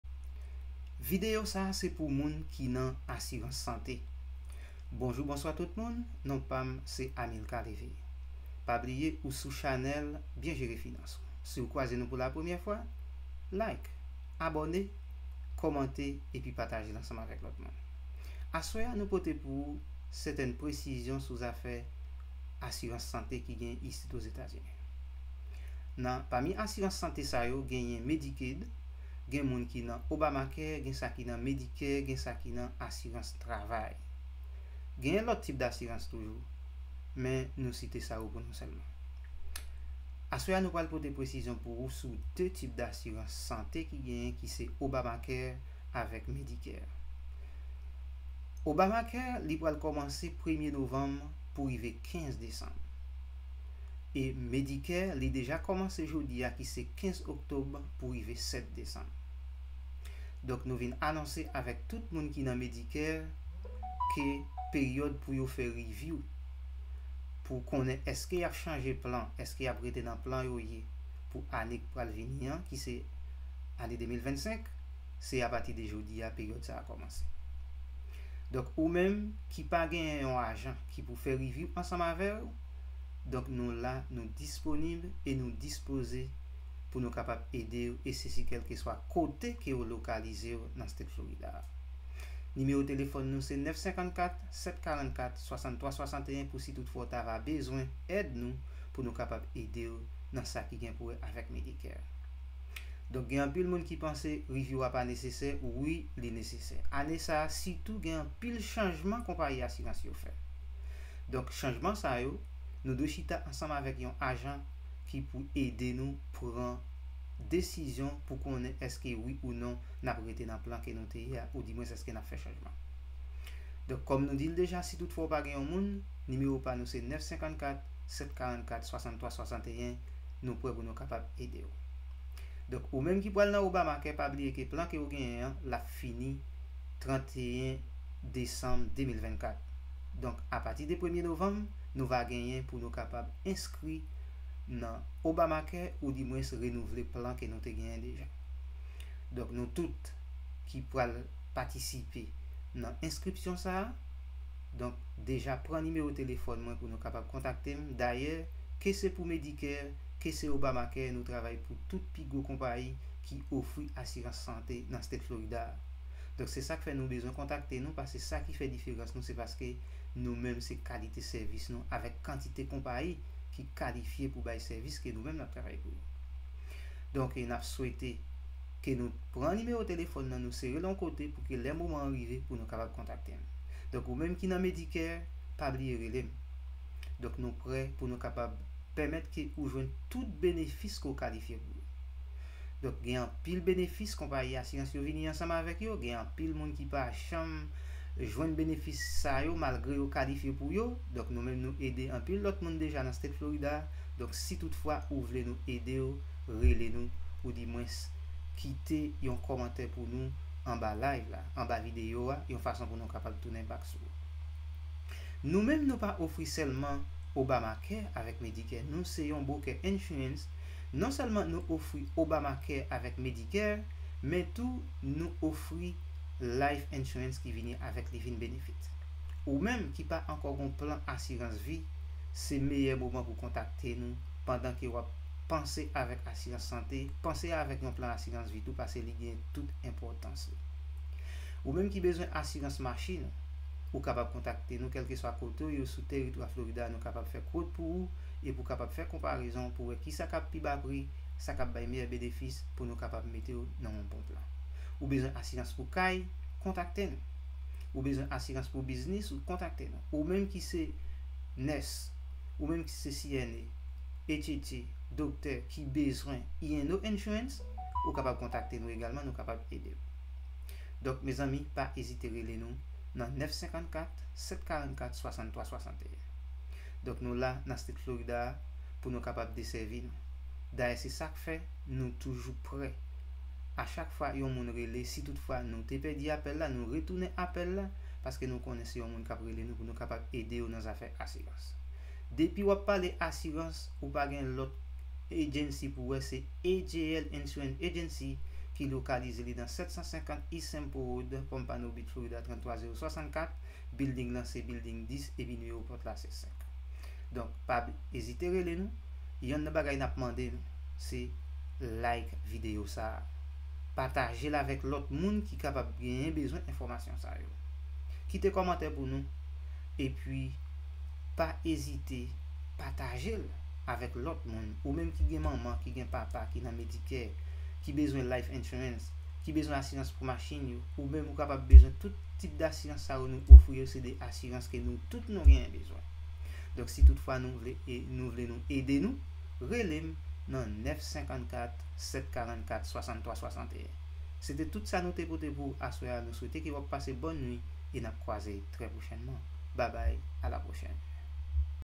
Asirans Sante (Medicare & Obamacare)
Vin aprann koman w kapab jwenn yon bon plan Medicare ak Obamacare pou ane 2025 lan.